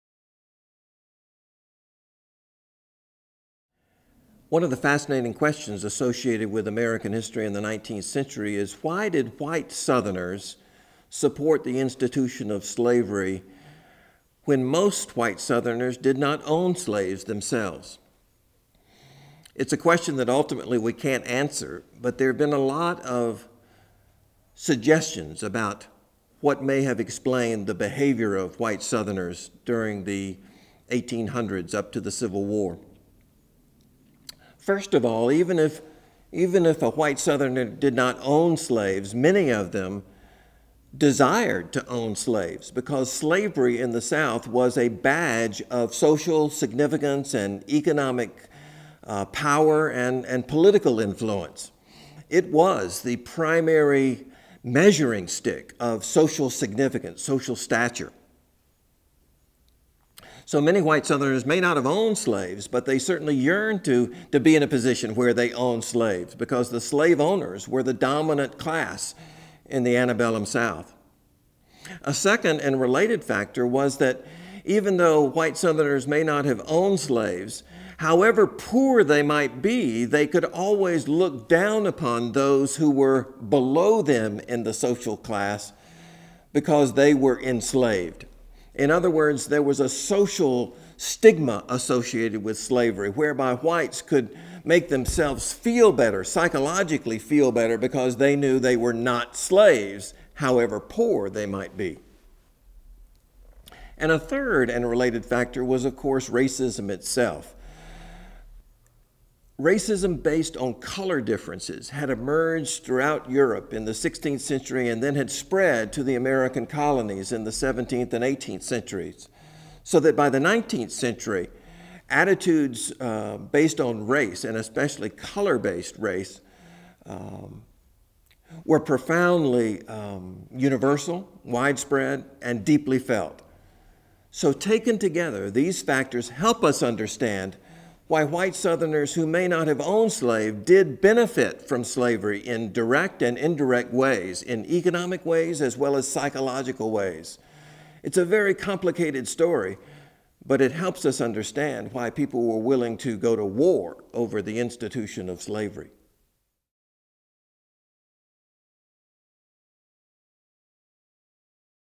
David Shi on why many non-slaveowning white southerners supported slavery
Interviews with David Shi, co-author of AMERICA: A NARRATIVE HISTORY

For more media and a transcript of this interview, visit the America StudySpace site: